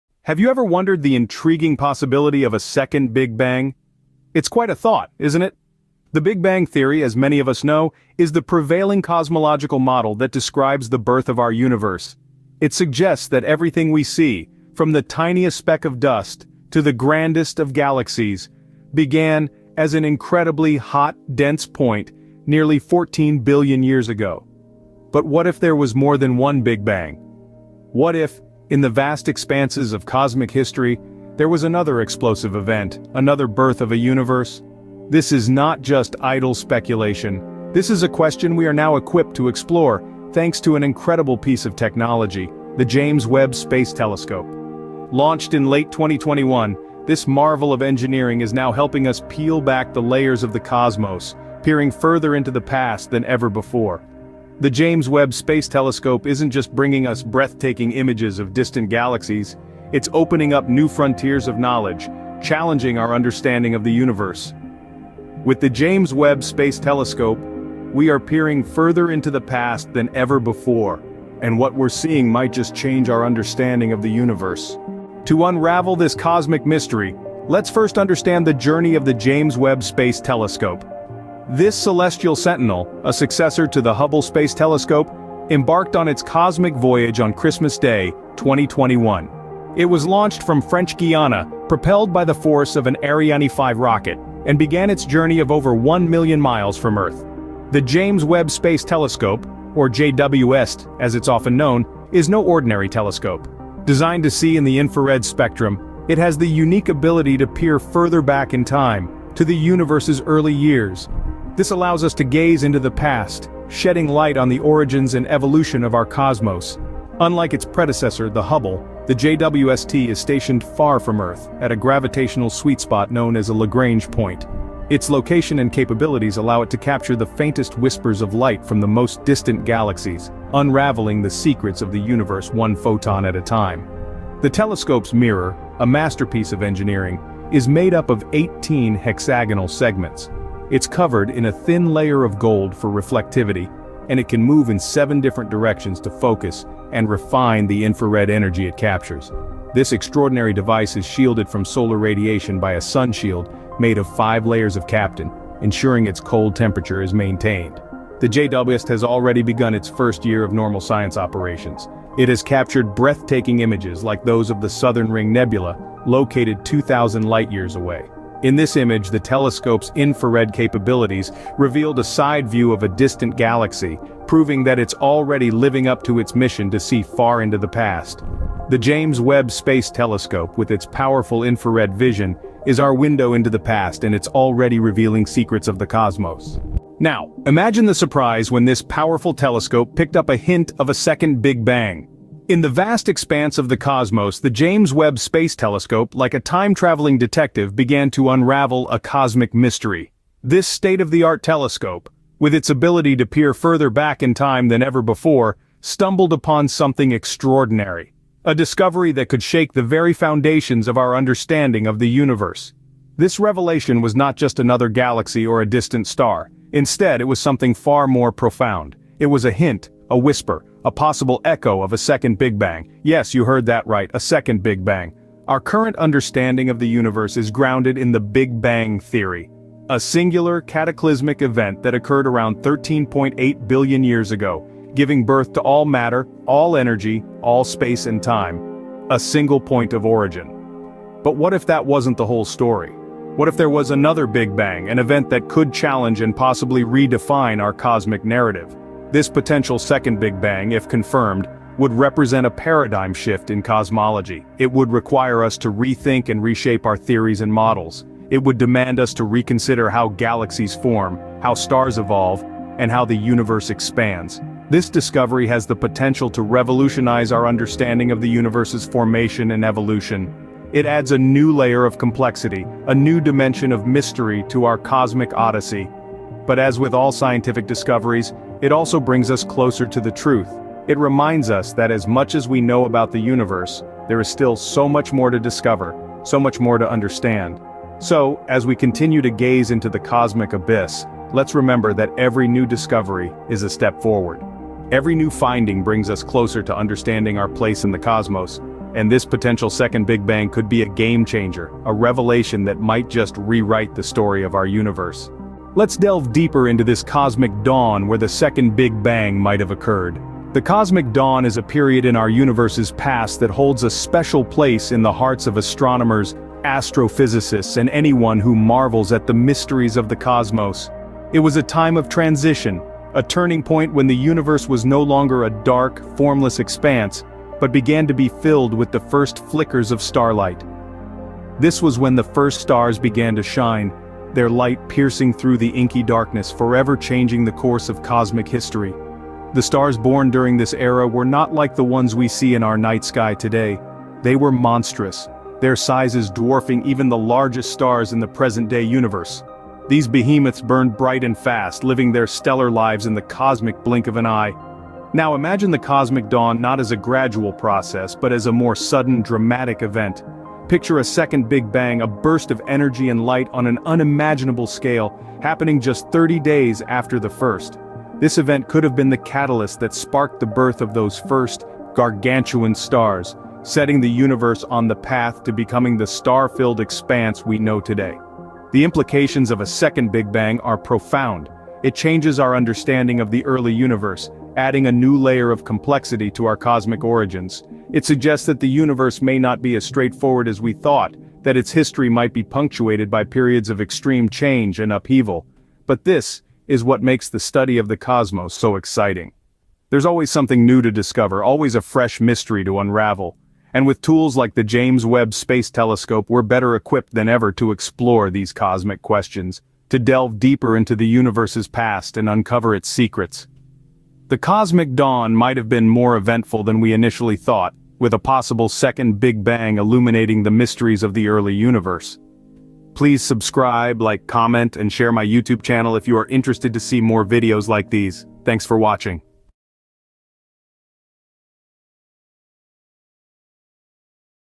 James Webb Hints Second Big Bang
Embark on a mind-bending cosmic odyssey with the James Webb Space Telescope, as it unveils a stunning revelation: a hint of another Big Bang, just 30 days after the initial explosion at Cosmic Dawn. This groundbreaking discovery challenges the very fabric of our understanding of the universe and beckons us to reevaluate the cosmic narrative.

At the heart of this revelation is the James Webb Telescope's ability to peer deep into the cosmic dawn, a time when the universe was in its infancy. The subtle clues detected by its sophisticated instruments suggest a phenomenon as profound as a second Big Bang, sending ripples through the fabric of spacetime.

Scientists theorize that this potential second explosion could be a cosmic echo, a reverberation of primordial forces unleashed in the early moments of the universe. The implications are staggering, raising questions about the nature of time, the expansion of the cosmos, and the fundamental forces that govern our reality.

The discovery opens a new chapter in astrophysics, inviting us to contemplate the existence of multiple cosmic epochs, each marked by its unique set of physical laws and cosmic dynamics. It challenges our perceptions of a linear cosmic evolution and invites us to consider a more complex and intricate narrative of the universe's history.

Join us on this celestial journey as we explore the ramifications of this revelation and its potential to reshape our understanding of existence itself. Could there be other cosmic cycles, each birthing a new universe in a cyclical dance of creation and destruction?

As we unravel the cosmic mysteries hinted at by the James Webb Space Telescope, we invite you to engage in the conversation. Share your thoughts, theories, and questions using SecondBigBang CosmicDawnDiscovery JamesWebbTelescope. 🌌🔭